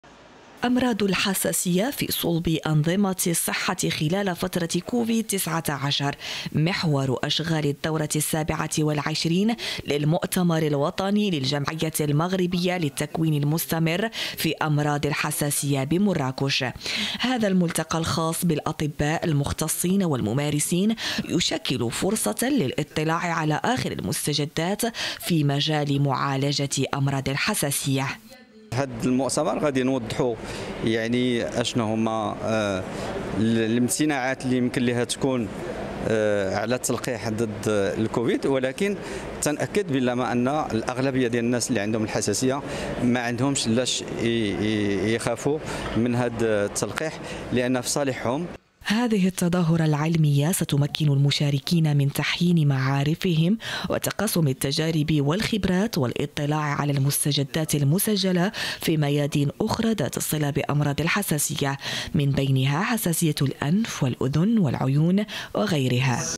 الحساسية وكوفيد 19 محور أشغال المؤتمر الوطني ال27 للجمعية المغربية للتكوين المستمر في أمراض الحساسية
يشكل موضوع "أمراض الحساسية في صلب أنظمة الصحة خلال فترة كوفيد 19"، محور أشغال الدورة ال27 للمؤتمر الوطني للجمعية المغربية للتكوين المستمر في أمراض الحساسية، التي انطلقت أشغالها، اليوم الخميس، بمراكش.

لمتابعتنا
لتحميل التطبيق
Android